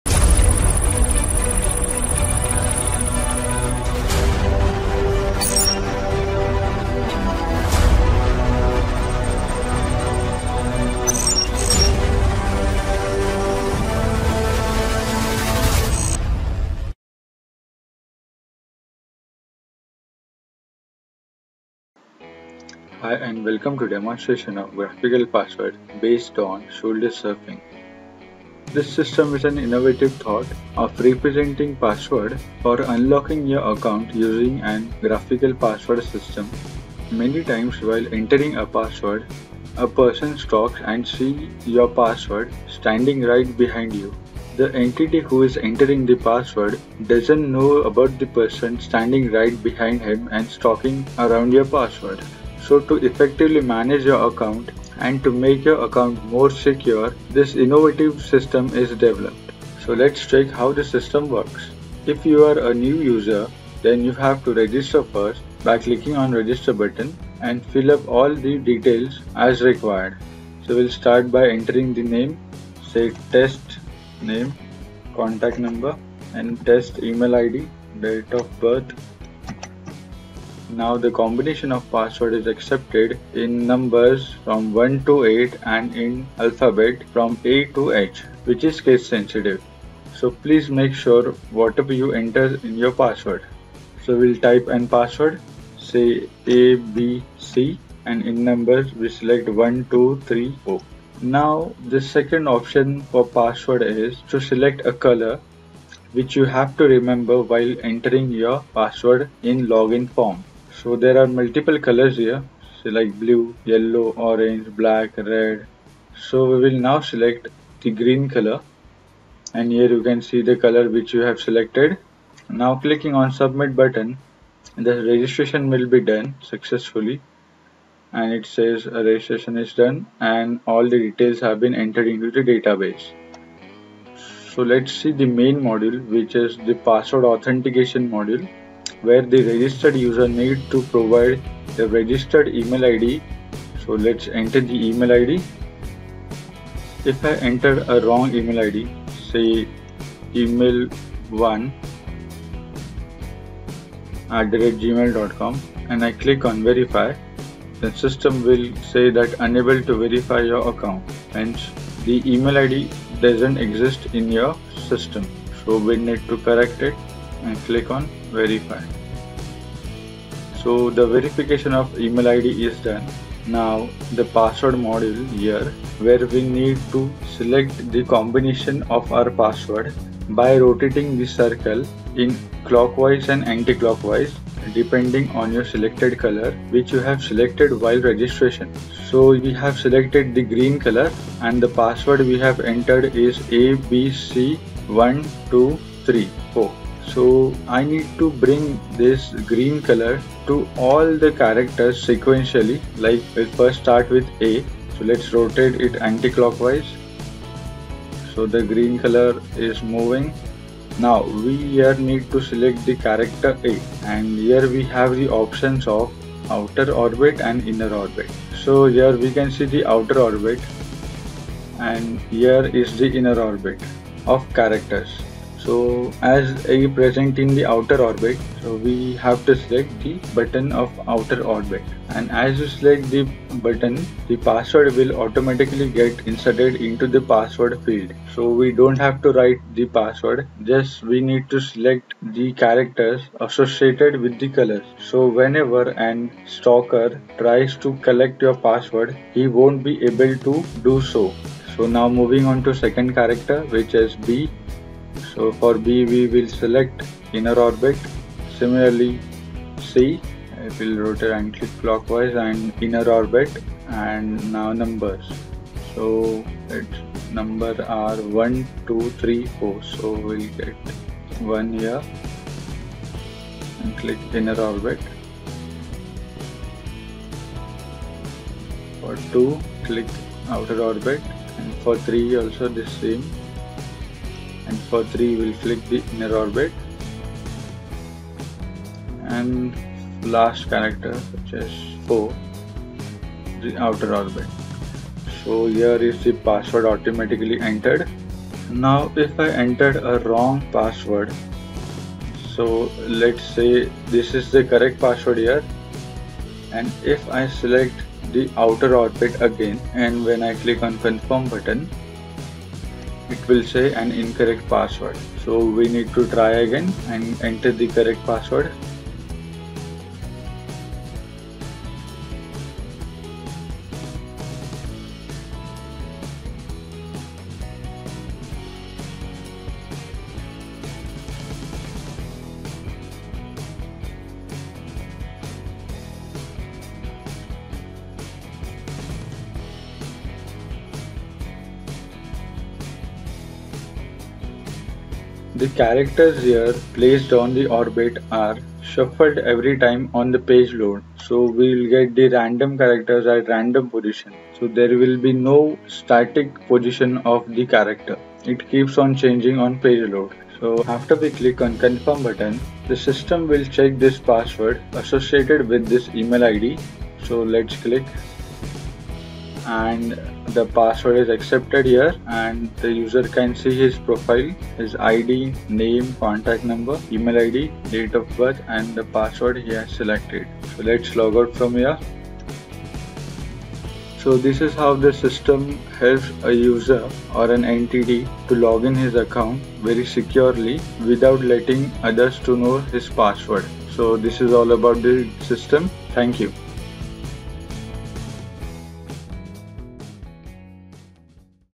Graphical Password Based to Avoid Shoulder Surfing
System allows for graphical password system to avoid shoulder surfing